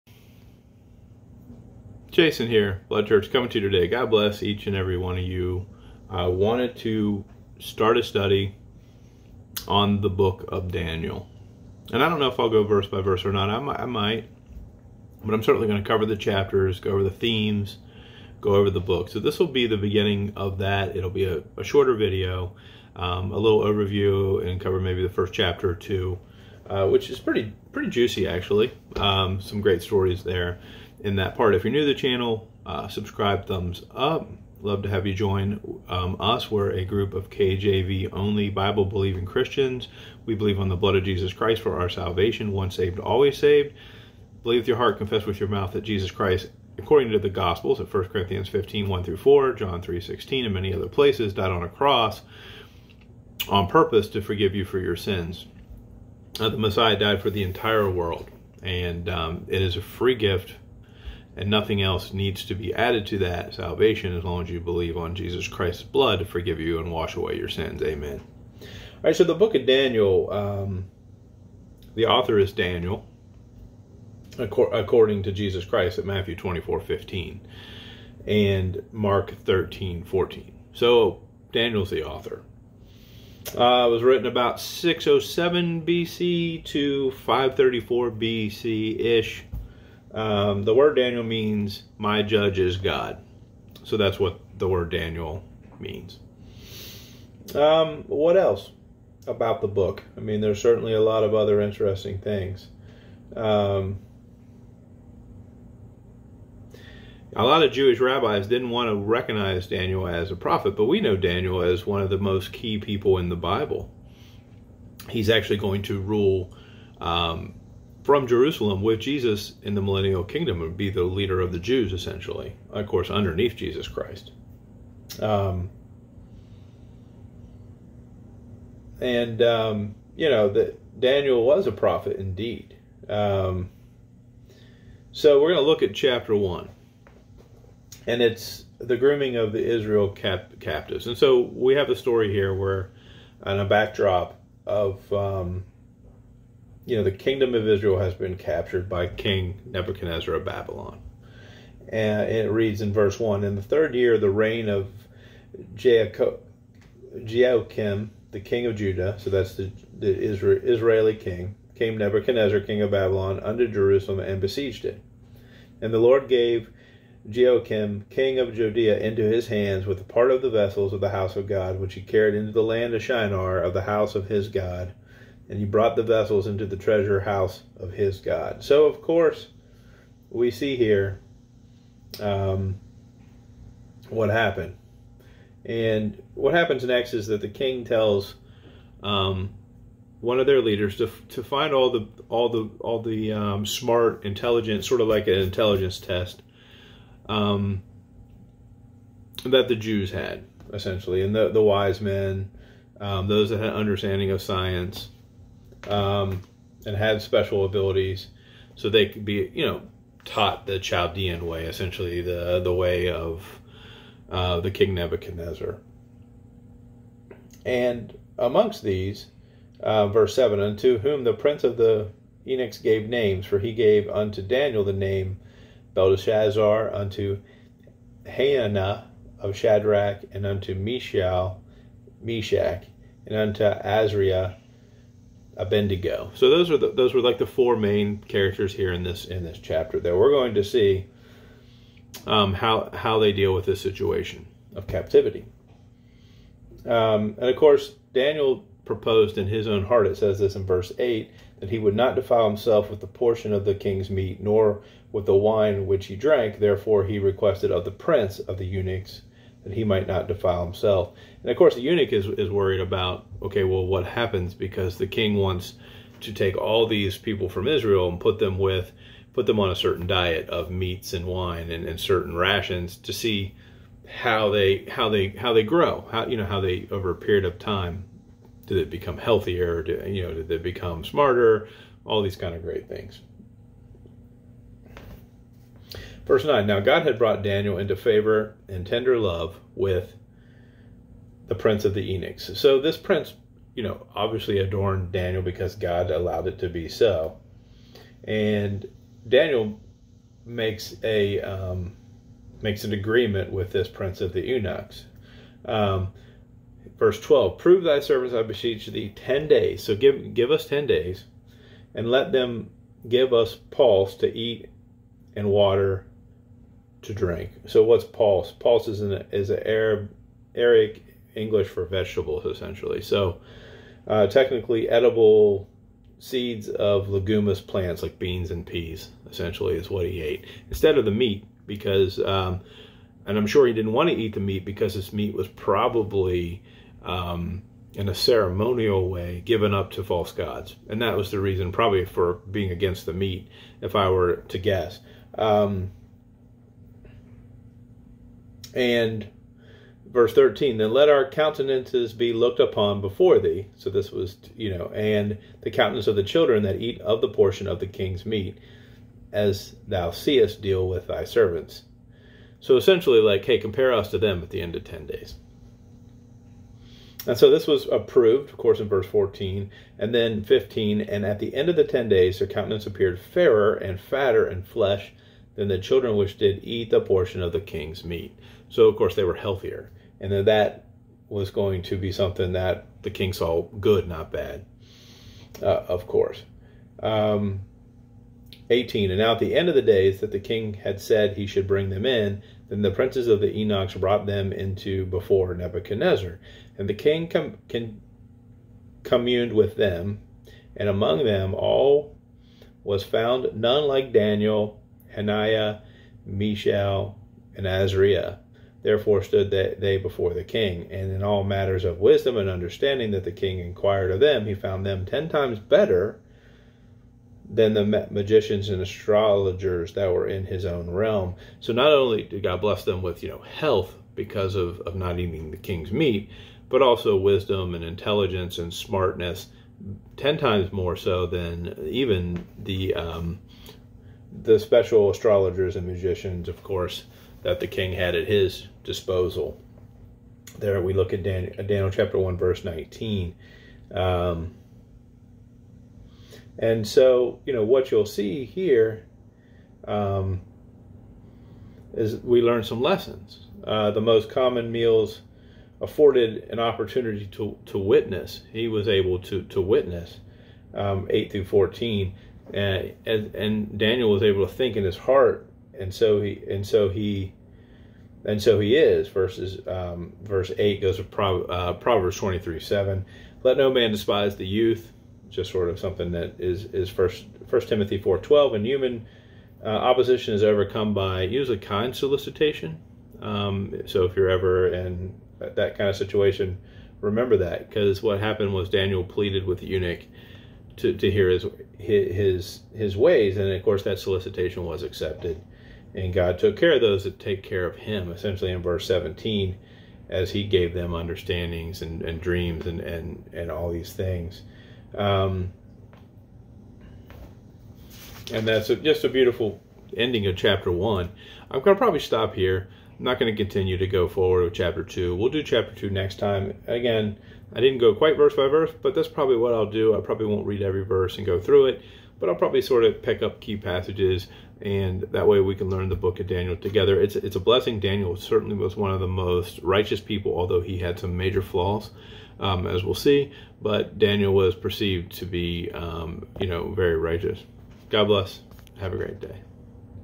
Daniel Book - captivity and persecution handled by God
Plus notice verses Jesus resurrection witnessed in verses 5-6 by over 500 Jews.

Romans 10: 9-10 confess with mouth and believe with heart for  salvation

Colossians 1:14 shows our redemption through blood of Jesus all sins past present and future all forgive

Ephesians 4:30 says we are saved (sealed) until the day we die or we are raptured